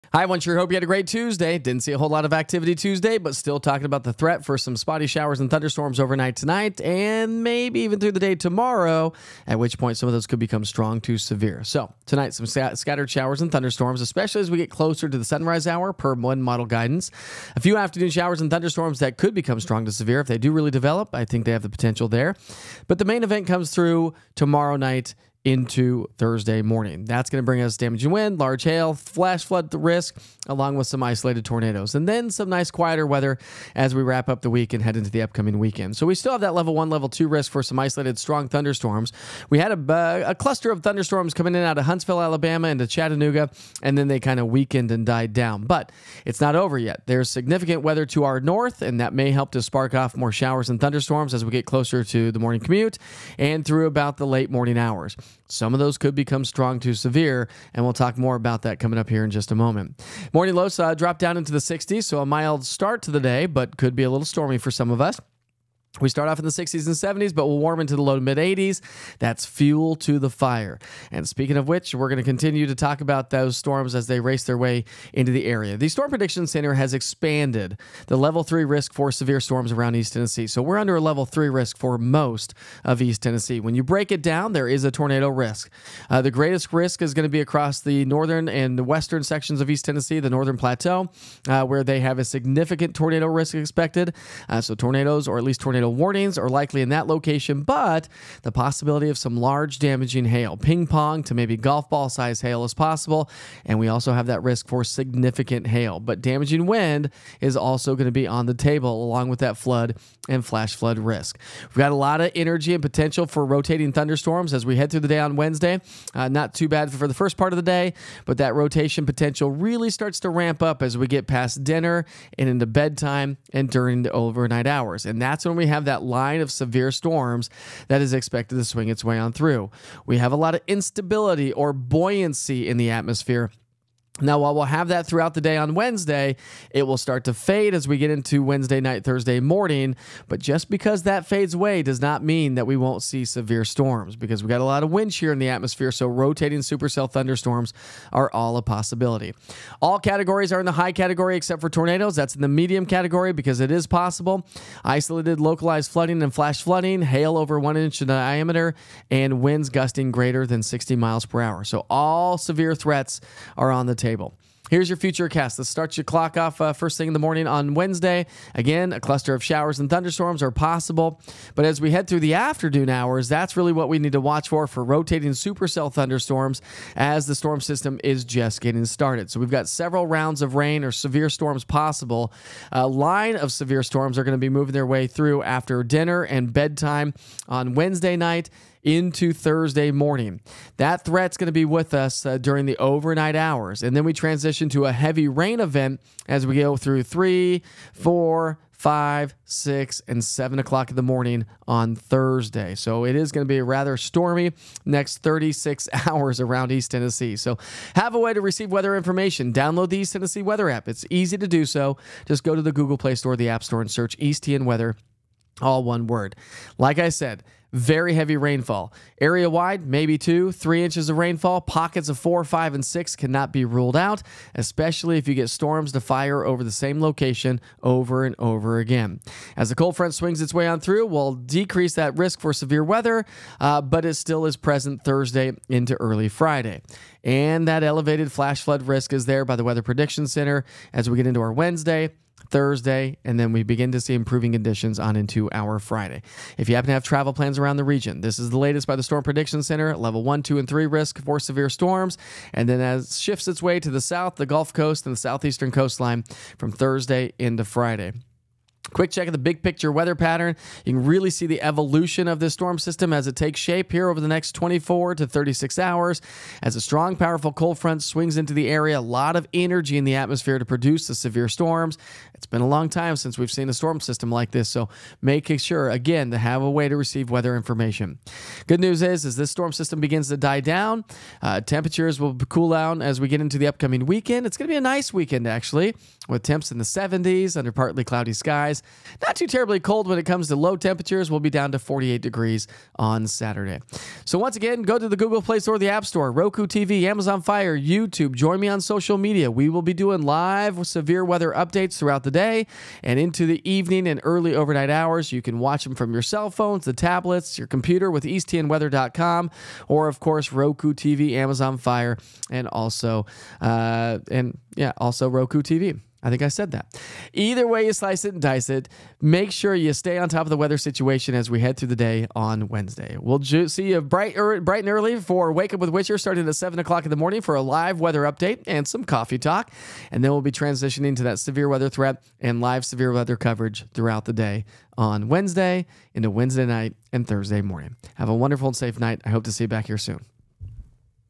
9pm - Tuesday, May 7, 2024
Here we go folks!  Severe weather is expected over the next 36 hours. Check in on the latest with Meteorologist Mike Witcher.